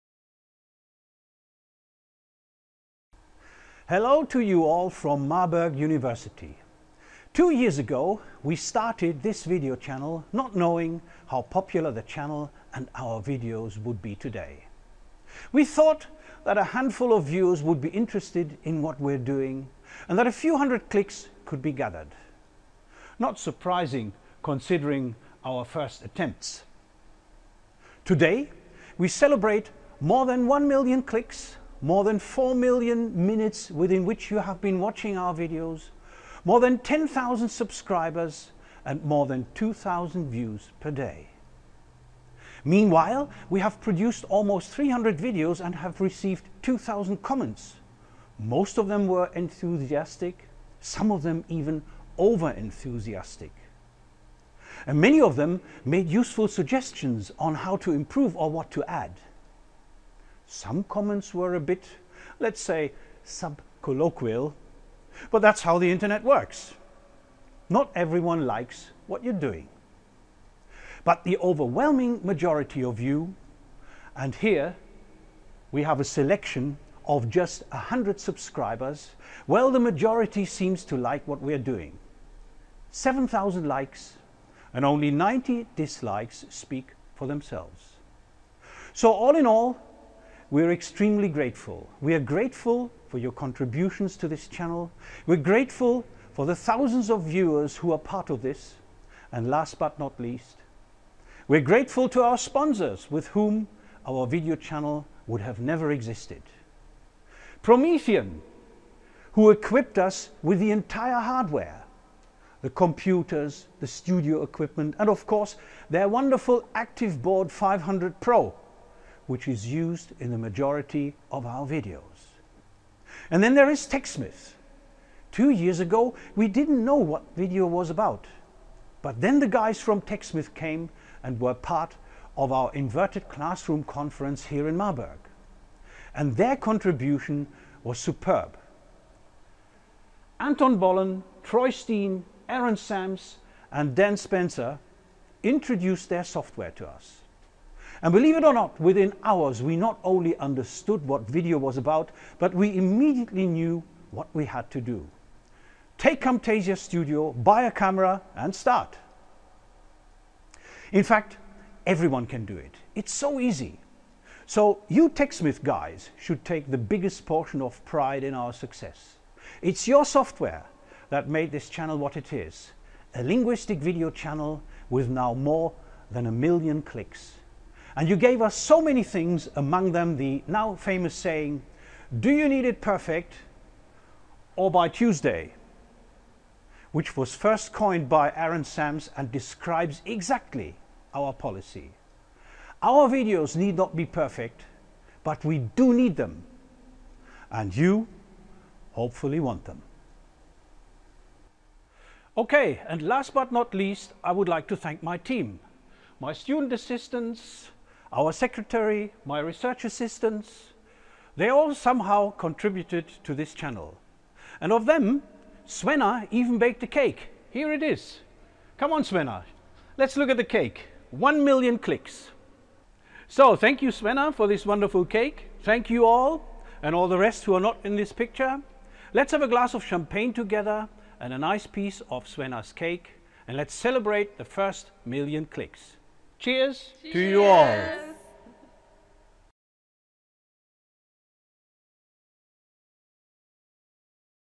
One Million Clicks
On 21 July 2014, we broke the 1 million click barrier in our linguistics YouTube channel. This is video was produced to celebrate this memorable event. When we started our channel 2 years ago, we never even imagined such a success: 1.000,000 clicks; 10,000 subscribers; 7,000 Likes; and only 90 Dislikes --- and ca. 4,000 new community members on the Virtual Linguistics Campus.

So thank you all, we will continue trying hard. We are here for you all.